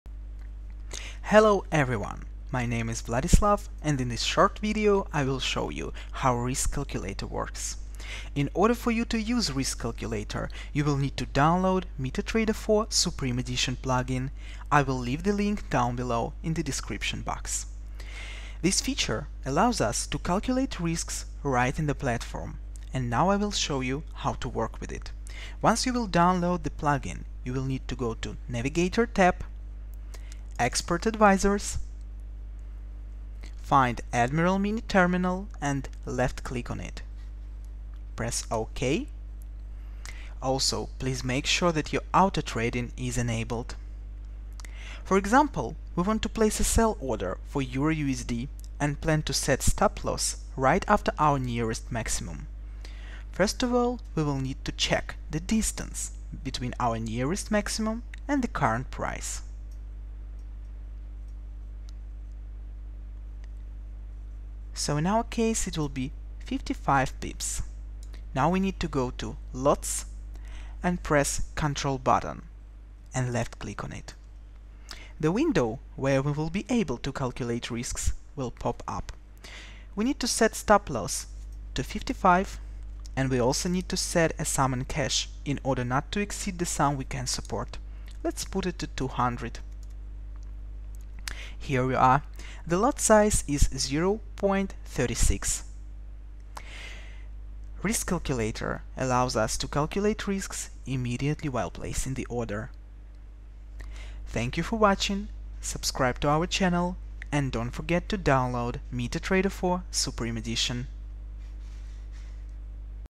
MT4 Supreme Edition: Risk calculator
This video will help you to understand how risk calculator works. This indicator allows you to monitor your risks while placing an order.